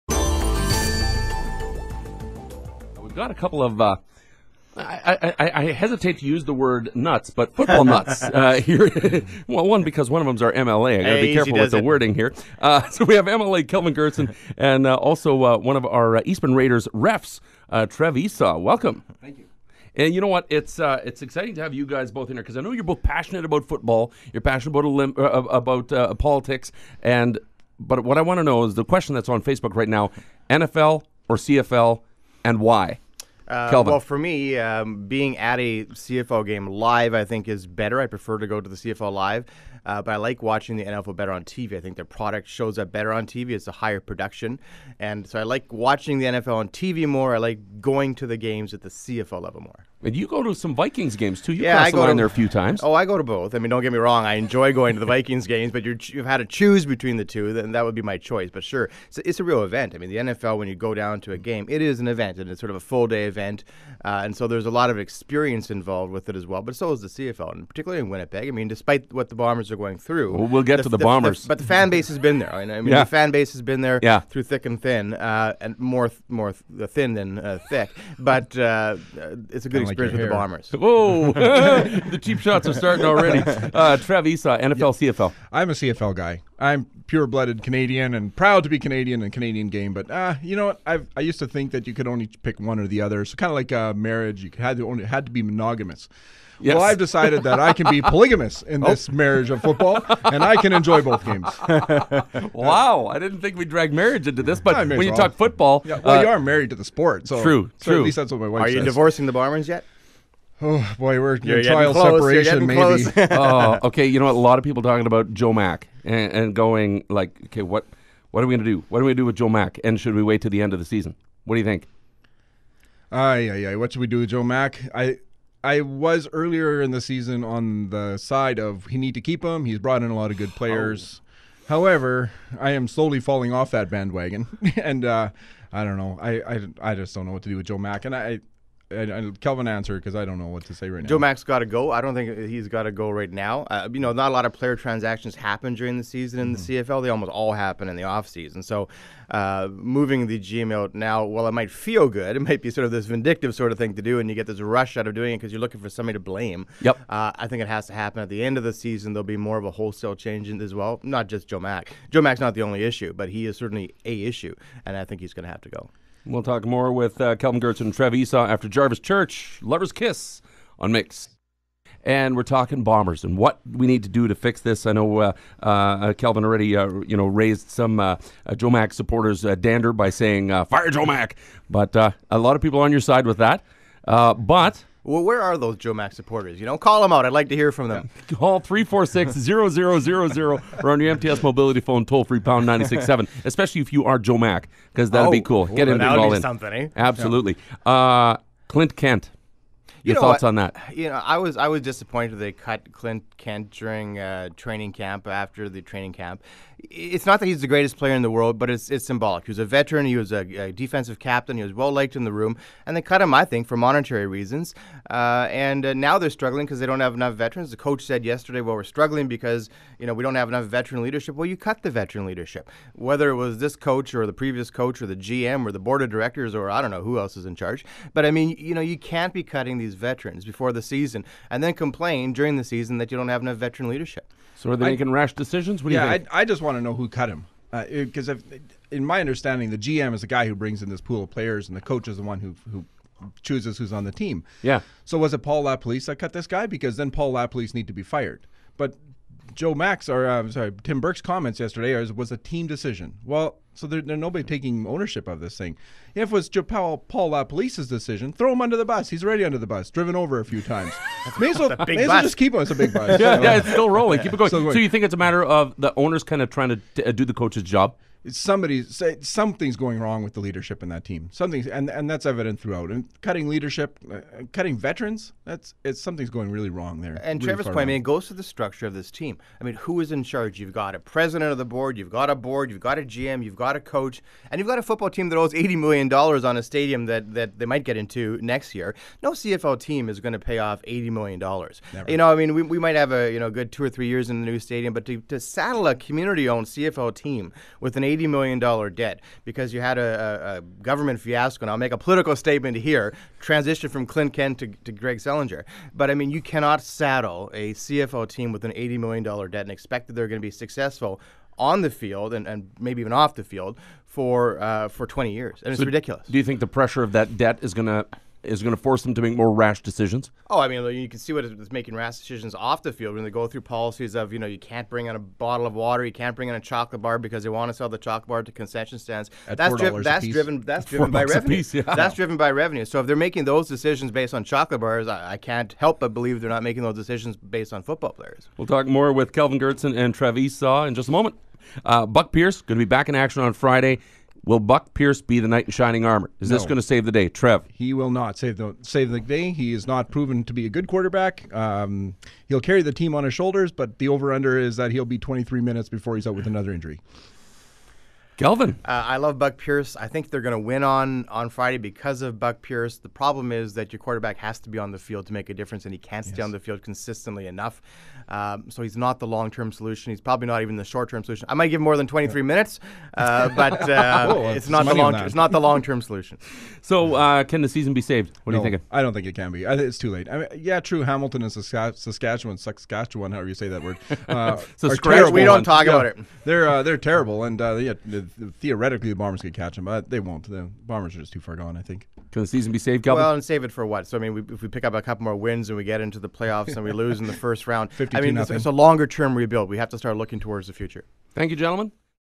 Kelvin Goertzen and Trev Esau Talk Blue Bombers
MLA Kelvin Goertzen and Minor Football Ref Trev Esau are both hardcore football fans. The topic of the Winnipeg Blue Bombers has a lot of fans riled up. What do Kelvin and Trev have to say about it?